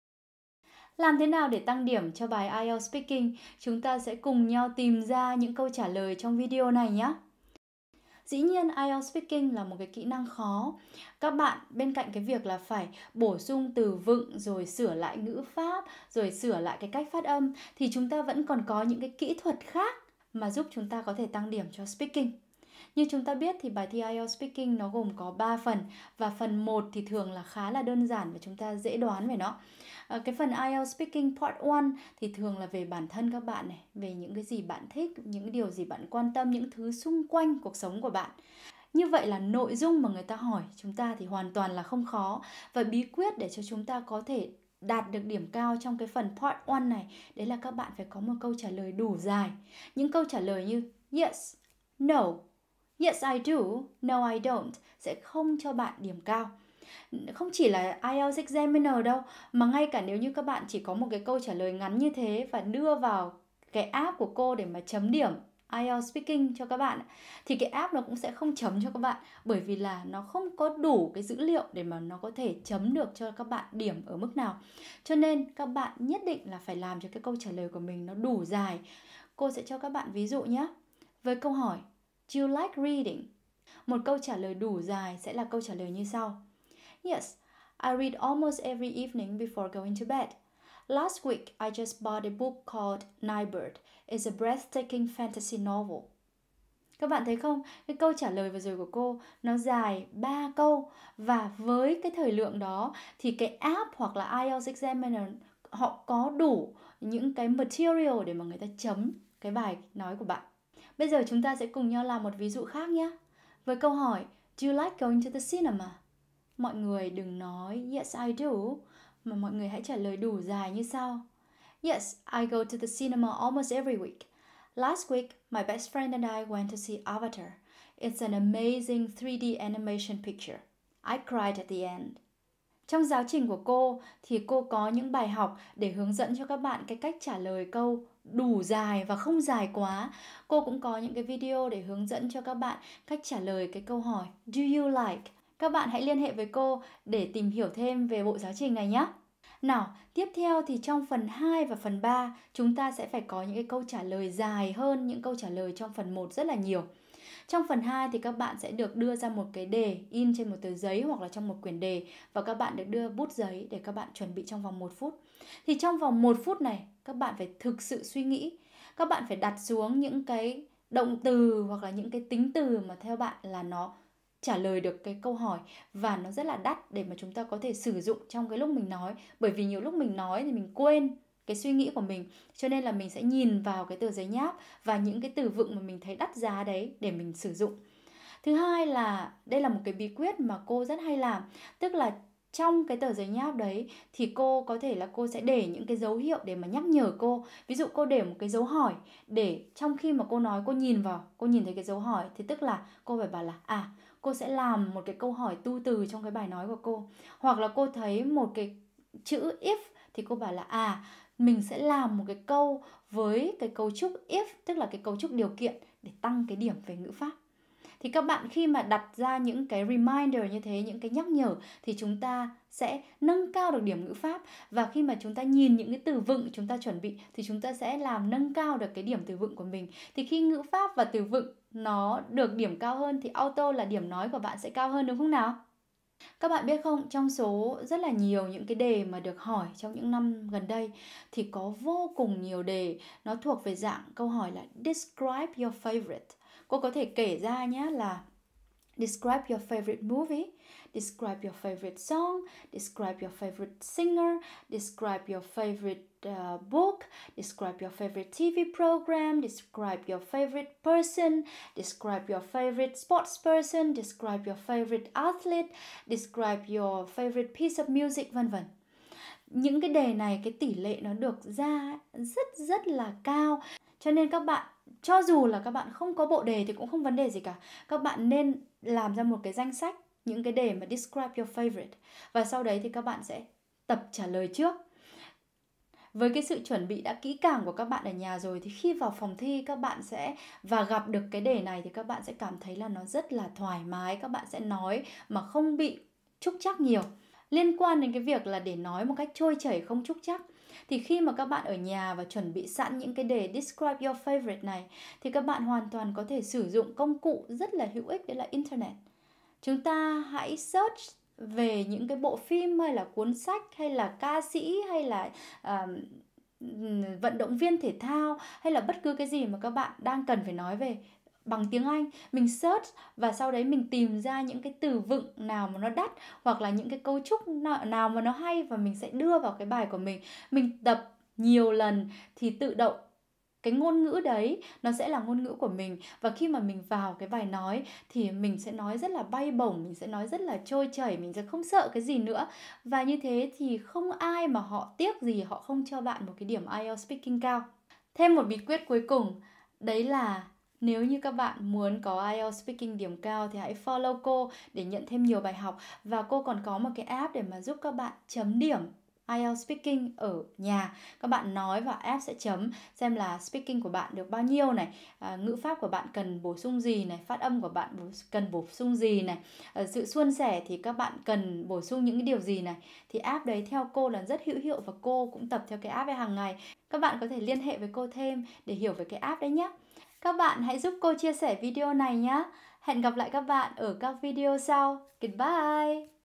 4 BÍ QUYẾT NHẤT ĐỊNH PHẢI BIẾT ĐỂ TĂNG ĐIỂM IELTS SPEAKING
4 BÍ QUYẾT NHẤT ĐỊNH PHẢI BIẾT ĐỂ TĂNG ĐIỂM IELTS SPEAKING (RẤT RẤT ĐƠN GIẢN)
Phần thi IELTS speaking luôn là phần thi khó với cả các cao thủ lẫn whoever-thủ. Nhưng nếu nắm được 4 bí quyết mà cô chia sẻ trong bài dưới đây thì khả năng chúng ta tăng được điểm cho phần speaking là HOÀN TOÀN CÓ THỂ.
Những bí quyết cô nói rất cụ thể trong video, nhưng ngắn gọn thì chúng liên quan đến:
✅ độ dài của câu trả lời: ngắn quá cũng hỏng mà nhiều quá cũng dở
✅làm gì trong 1 phút chuẩn bị (chuẩn bị đúng có thể TĂNG ĐIỂM ĐÁNG KỂ)
✅ không có bộ đề để luyện thì luyện như nào để vẫn có khả năng trúng đề cao
✅ cách tự chấm điểm nói chính xác
Những bí quyết này không liên quan đến việc phải ôn luyện lại ngữ pháp hay phải cải thiện phát âm (vì hai cái này là hai điều dĩ nhiên phải làm và ai cũng biết, chưa kể là cải thiện được là điều rất khó).
Mọi người hãy xem kỹ những lời khuyên này của cô, và chia sẻ bài này cho các bạn của mình nữa nhé.